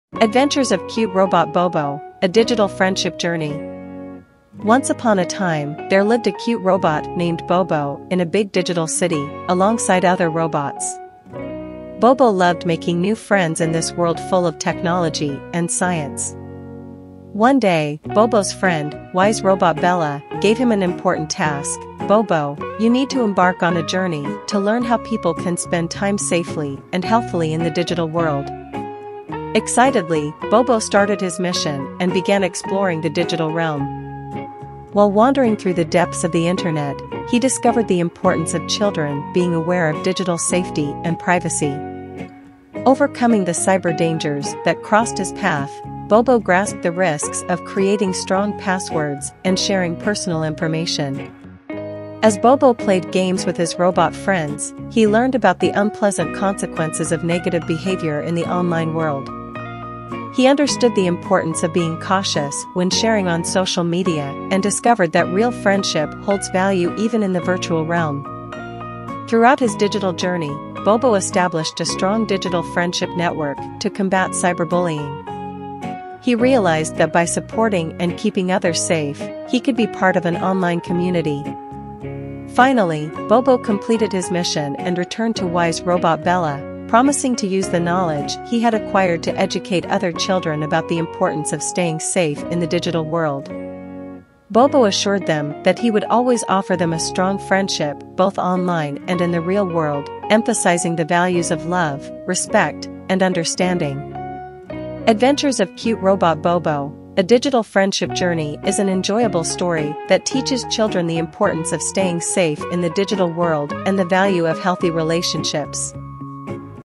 Adventures of Cute Robot Bobo A Digital Friendship Journey
Once upon a time, there lived a cute robot named Bobo in a big digital city, alongside other robots. Bobo loved making new friends in this world full of technology and science. One day, Bobo's friend, Wise Robot Bella, gave him an important task: "Bobo, you need to embark on a journey to learn how people can spend time safely and healthily in the digital world."

Excitedly, Bobo started his mission and began exploring the digital realm. While wandering through the depths of the internet, he discovered the importance of children being aware of digital safety and privacy. Overcoming the cyber dangers that crossed his path, Bobo grasped the risks of creating strong passwords and sharing personal information.

As Bobo played games with his robot friends, he learned about the unpleasant consequences of negative behavior in the online world. He understood the importance of being cautious when sharing on social media and discovered that real friendship holds value even in the virtual realm.

Throughout his digital journey, Bobo established a strong digital friendship network to combat cyberbullying. He realized that by supporting and keeping others safe, he could be part of an online community.

Finally, Bobo completed his mission and returned to Wise Robot Bella, promising to use the knowledge he had acquired to educate other children about the importance of staying safe in the digital world. Bobo assured them that he would always offer them a strong friendship both online and in the real world, emphasizing the values of love, respect, and understanding.

"Adventures of Cute Robot Bobo: A Digital Friendship Journey" is an enjoyable story that teaches children the importance of staying safe in the digital world and the value of healthy relationships.